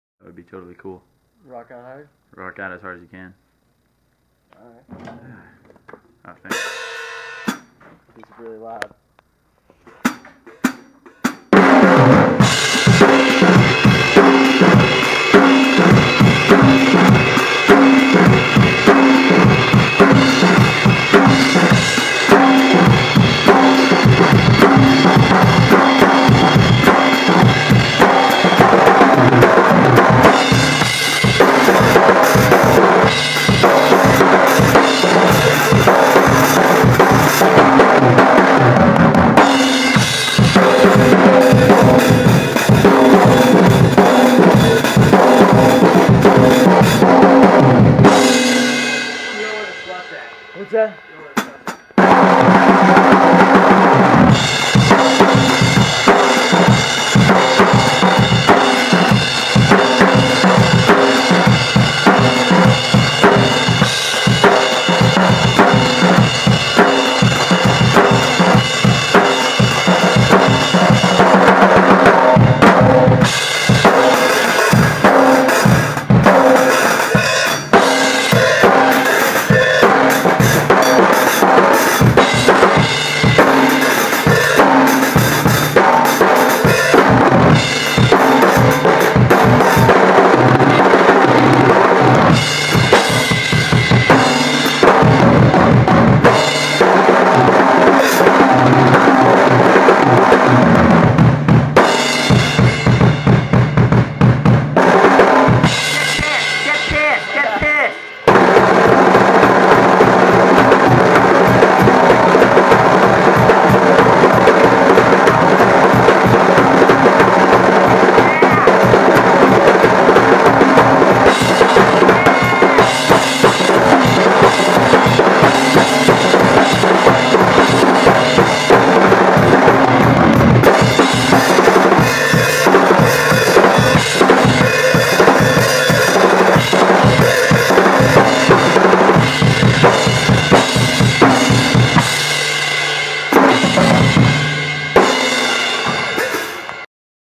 Dave Rocking Out Really Hard
Playin some drums in our rehearsal studio.